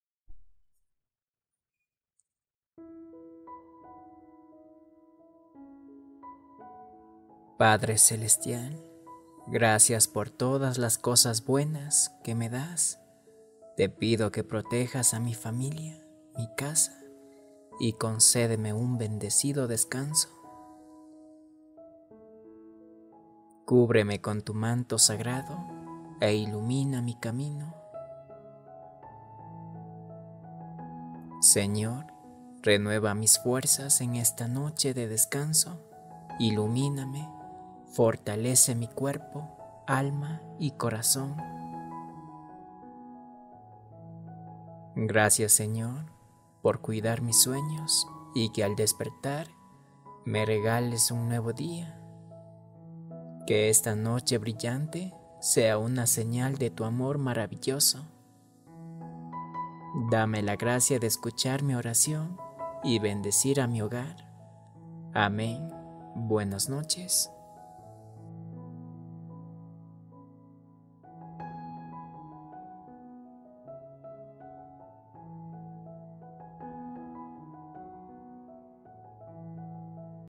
Buenas Noches Hasta Mañana con Mensaje Hermoso Para Ti Un Video de hasta Mañana
Buenas Noches Dulces Sueños
Padre Celestial, Gracias por todas las cosas buenas que me das, te pido que protejas a mi familia, mi casa y concédeme un bendecido descanso.